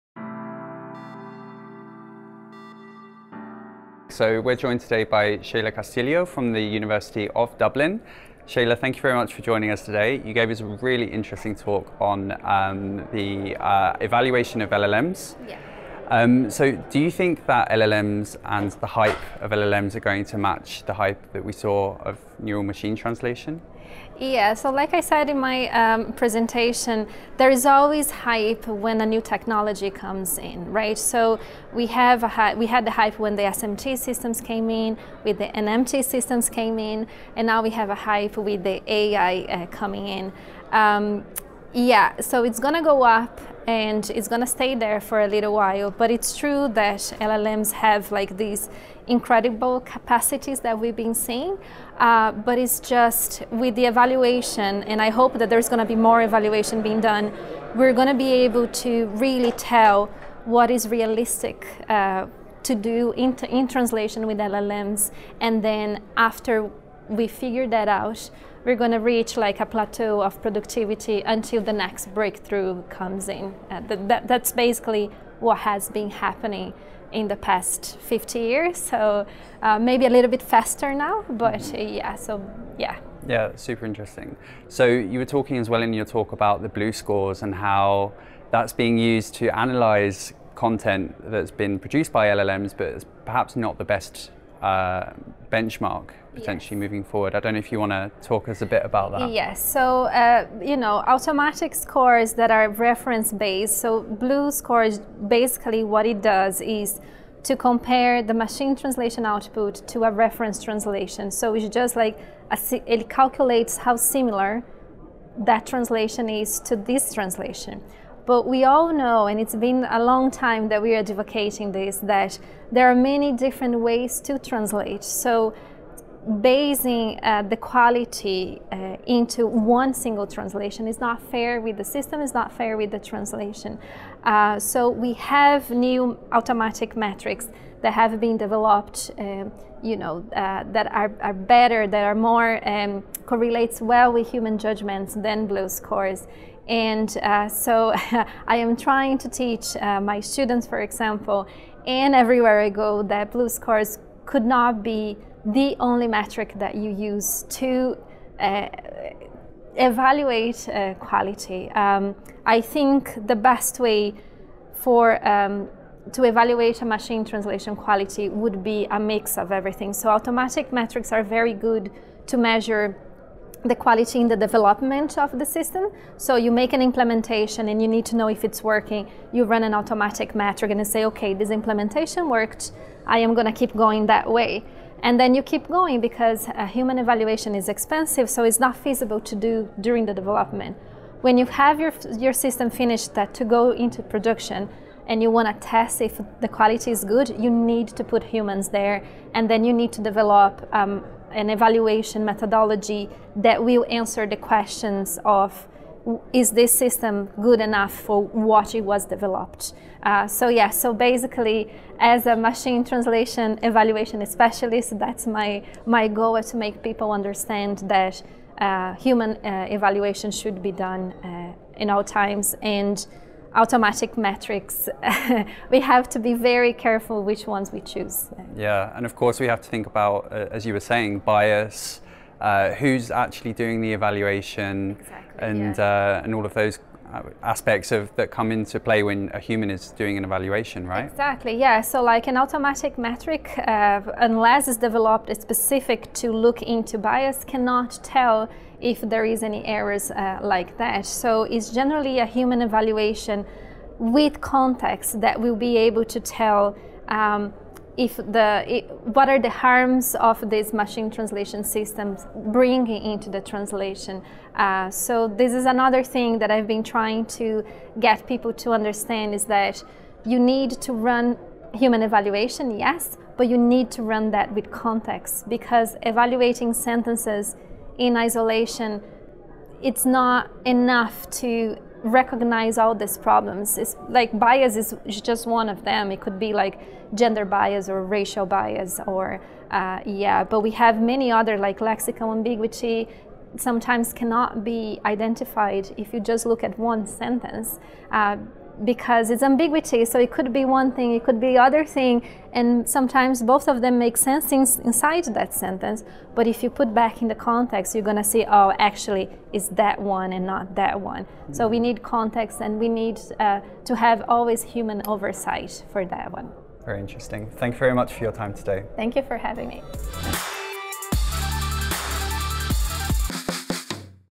Dr. Sheila Castilho, Assistant Professor, Dublin City University at SlatorCon Zurich 2023
At SlatorCon Zurich 2023, Slator's Alex Edwards interviews Dublin City University's Dr. Sheila Castilho on the hype of large language models (LLMs) and evaluating LLMs.